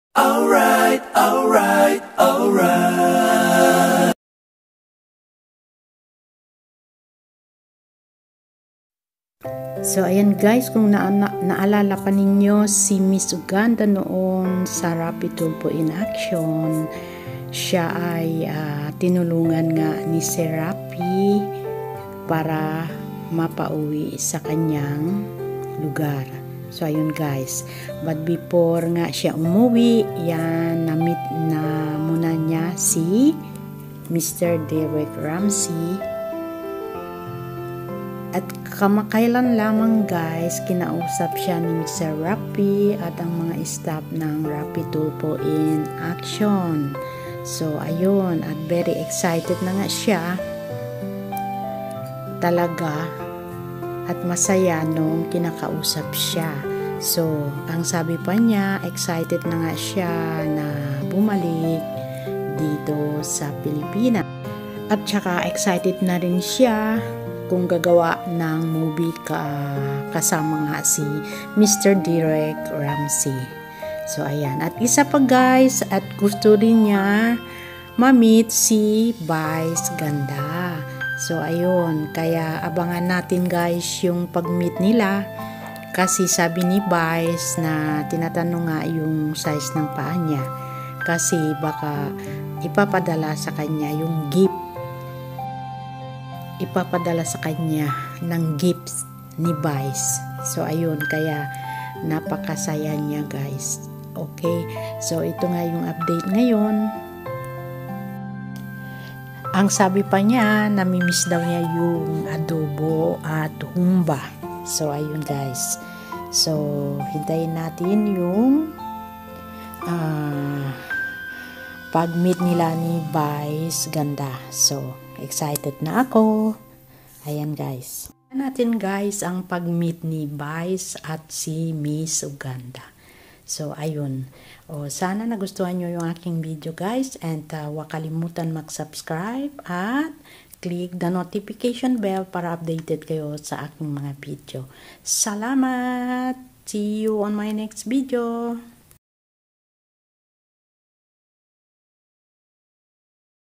UPDATE MISS UGANDA/EXCITED BUMALIK SA PILIPINAS/OFW VLOG BY MARY FRANCISCO
Kamakailan lamang kinausap night Sir Raffy at mag staff ng Raffy Tulfo in Action si Ms. Uganda. Masaya at excited siya bumalik sa pilipinas upang makita muli sis Mr. Derek. Hope you like this video. Enjoy watching.

Photos here is not mine.